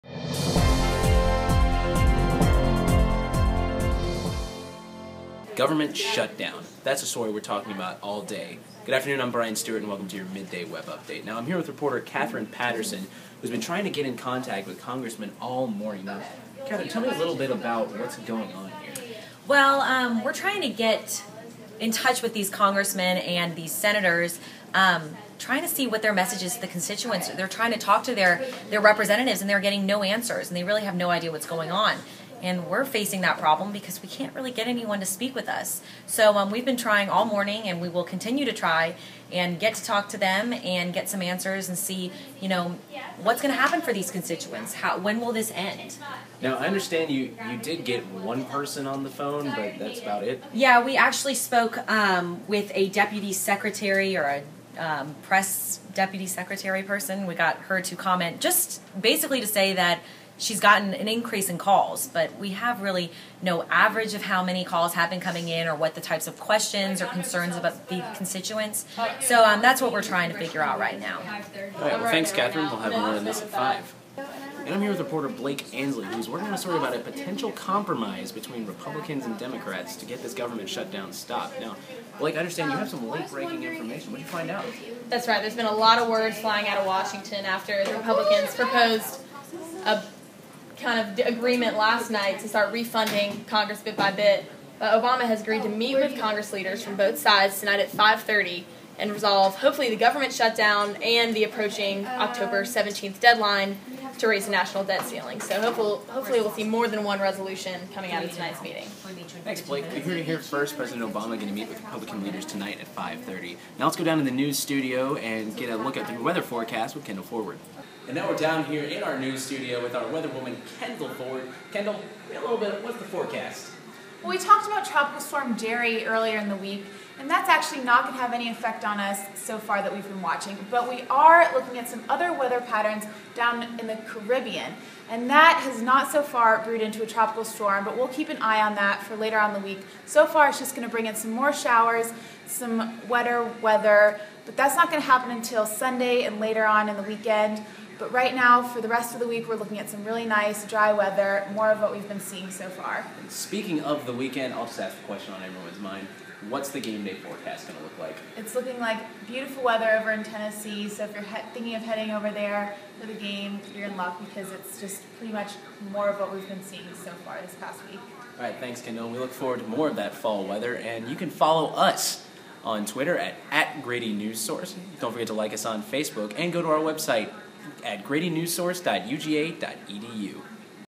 Afternoon Webcast 10/2
Brian Stewart checks in with our government shutdown reporters, Catherine Patterson and Blake Ansley, and our weather woman, Kendall Forward.